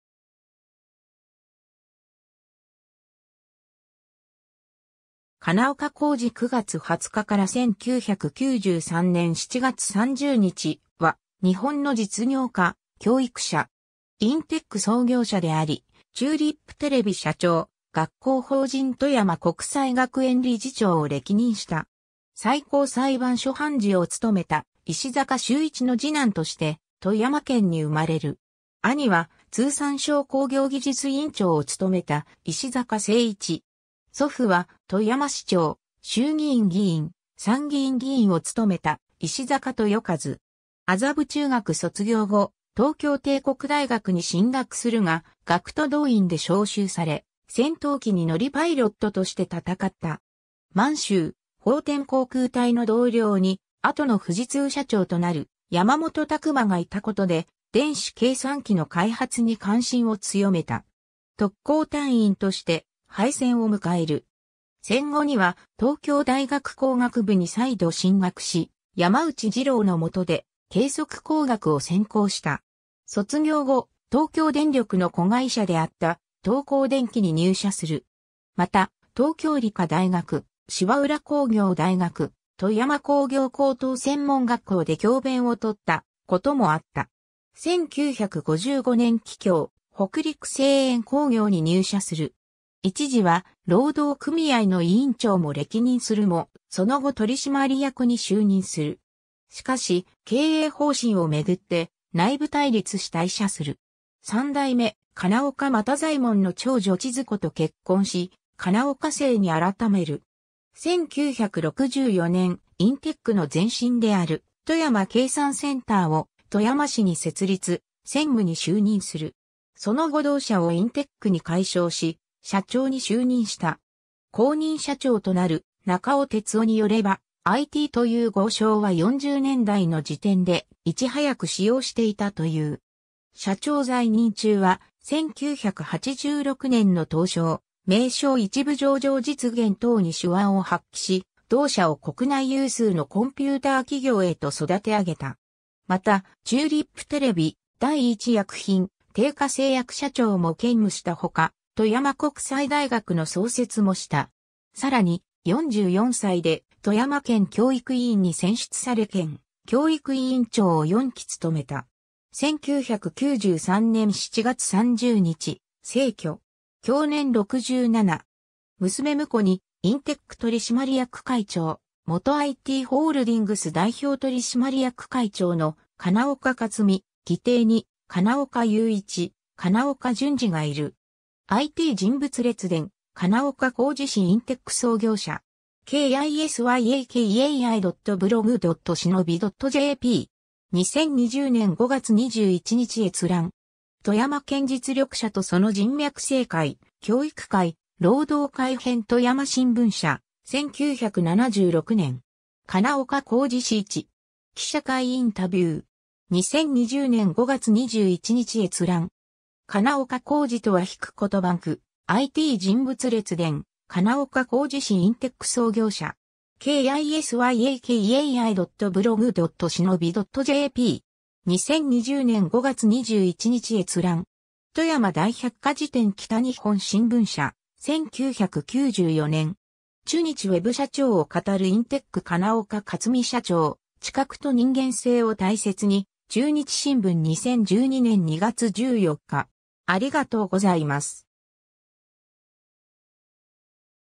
金岡幸二
金岡 幸二（かなおか こうじ、旧姓：石坂、1925年（大正14年）9月20日 - 1993年（平成5年）7月30日）は日本の実業家・教育者。
インテック創業者であり、チューリップテレビ社長、学校法人富山国際学園理事長を歴任した。
 最高裁判所判事を務めた石坂修一の次男として富山県に生まれる。
兄は通産省工業技術院長を務めた石坂誠一。
祖父は、富山市長、衆議院議員、参議院議員を務めた石坂豊一。
 麻布中学卒業後、東京帝国大学に進学するが学徒動員で召集され、戦闘機に乗りパイロットとして戦った。
満州・奉天航空隊の同僚に後の富士通社長となる山本卓眞がいたことで電子計算機の開発に関心を強めた。
特攻隊員として敗戦を迎える。
戦後には東京大学工学部に再度進学し、山内二郎のもとで計測工学を専攻した。
卒業後、東京電力の子会社であった東光電気に入社する。
また、東京理科大学、芝浦工業大学、富山工業高等専門学校で教鞭を取ったこともあった。
 1955年（昭和30年）帰郷、北陸製塩工業に入社する。
一時は労働組合の委員長も歴任するも、その後取締役に就任する。
しかし、経営方針をめぐって内部対立し退社する。
3代目金岡又左衛門の長女千鶴子と結婚し、金岡姓に改める。
 1964年（昭和39年）、インテックの前身である富山計算センターを富山市に設立、専務に就任する。
その後同社をインテックに改称し、社長に就任した。
後任社長となる中尾哲雄によれば、「IT」という語を昭和40年代の時点でいち早く使用していたという。
 社長在任中は、1986年（昭和61年）の東証、名証1部上場実現等に手腕を発揮し、同社を国内有数のコンピューター企業へと育て上げた。
また、チューリップテレビ、第一薬品、テイカ製薬社長も兼務したほか、富山国際大学の創設もした。
さらに、44歳で富山県教育委員に選出され県教育委員長を4期務めた。
 1993年（平成5年）7月30日、逝去。
享年67。
 娘婿にインテック取締役会長、元ITホールディングス代表取締役会長の金岡克己、義弟に金岡祐一、金岡純二がいる。
 ^ “ＩＴ人物列伝 金岡幸二氏　インテック創業者”. kisyakai.blog.shinobi.jp. 2020年5月21日閲覧。
 ^ 『富山県 実力者とその人脈 政界、教育界、労働界編 』富山新聞社、 1976年。
 ^ “金岡幸二氏（付中尾哲雄氏）①”. 記者会インタビュー. 2020年5月21日閲覧。
 ^ 金岡幸二とは - コトバンク ^ “ＩＴ人物列伝 金岡幸二氏　インテック創業者”. kisyakai.blog.shinobi.jp. 2020年5月21日閲覧。
 ^ 『富山大百科事典』 北日本新聞社、 1994年。
 ^ 「CHUNICHI Web 社長を語る インテック（富山市）金岡克己社長（55）知覚と人間性を大切に」 『中日新聞』 2012年2月14日